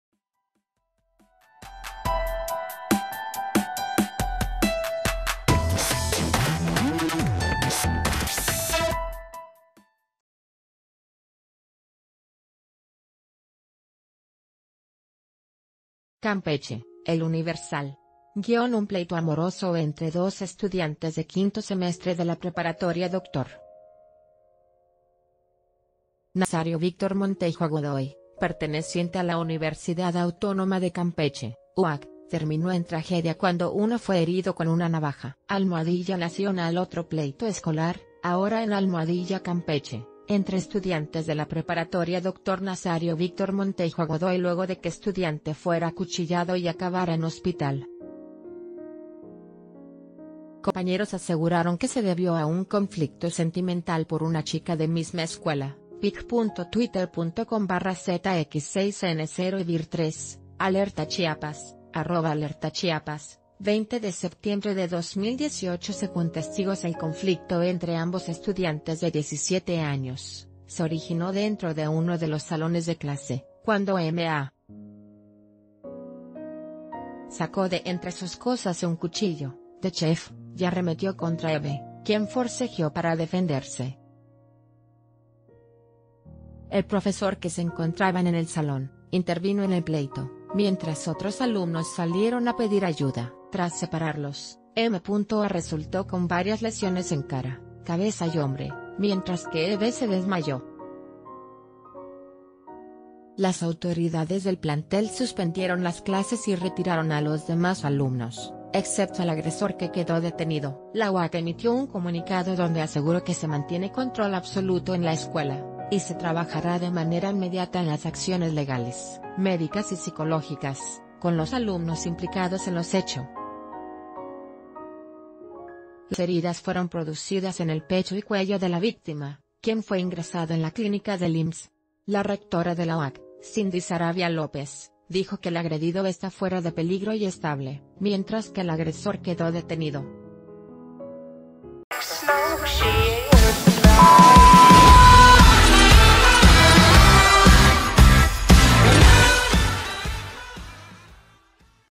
Riña estudiantil deja un herido con arma blanca - El Diario de Yucatán
CAMPECHE (El Universal).- Un pleito amoroso entre dos estudiantes de quinto semestre de la preparatoria Dr. Nazario Víctor Montejo Godoy, perteneciente a la Universidad Autónoma de Campeche (UAC), terminó en tragedia cuando uno fue herido con una navaja. Nacional Otro pleito escolar, ahora en Camp...